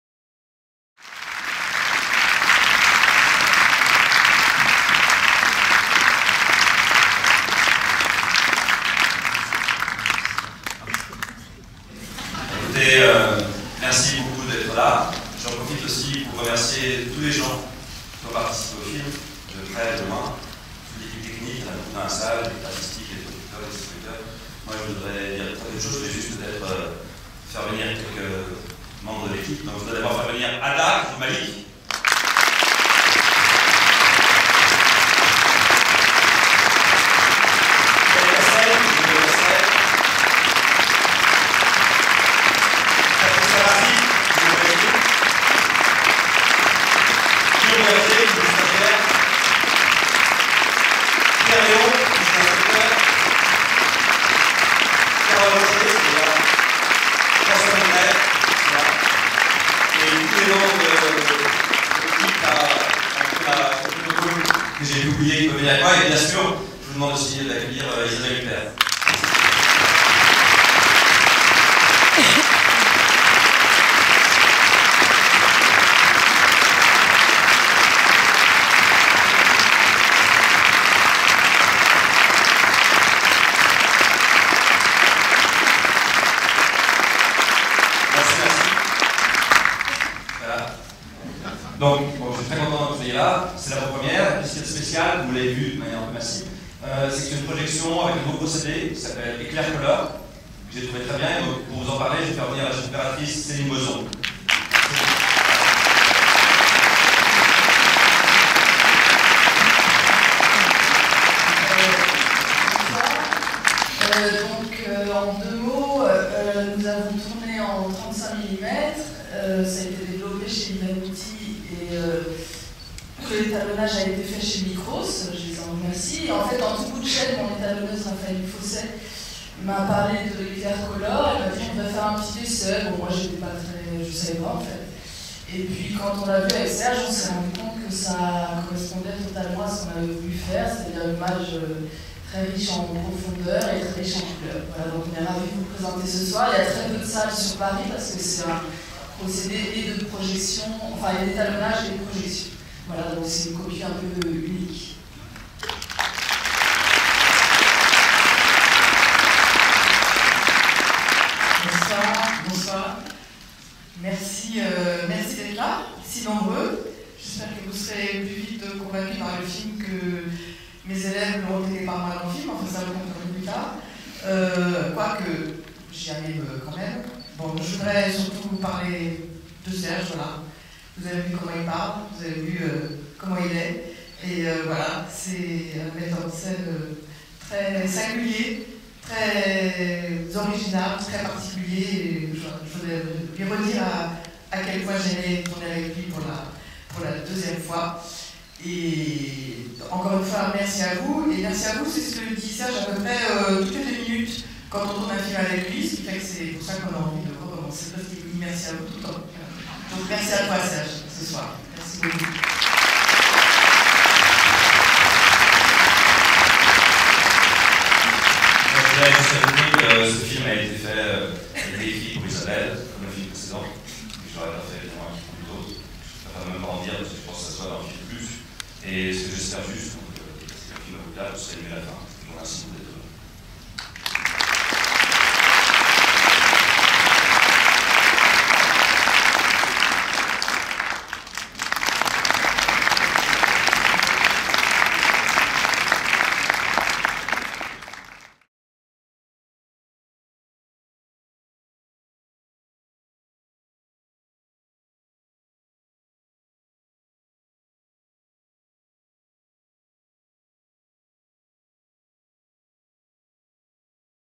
Madame Hyde - Serge Bozon, Isabelle Huppert (Gaumont Opéra, 27/03/2018)
Le réalisateur Serge Bozon a présenté son film "Madame Hyde" en avant-première au cinéma Gaumont Opéra le mardi 27 mars 2018, en présence de son équipe dont Isabelle Huppert et la chef-opératrice Céilne Bozon et les producteurs David Thion et Philippe Martin. Un film produit par Les Films Pelléas, Arte France Cinéma en coproduction avec Frakas Productions, distribué en France par Haut et Court. Le film est sorti au cinéma le 28 mars 2018.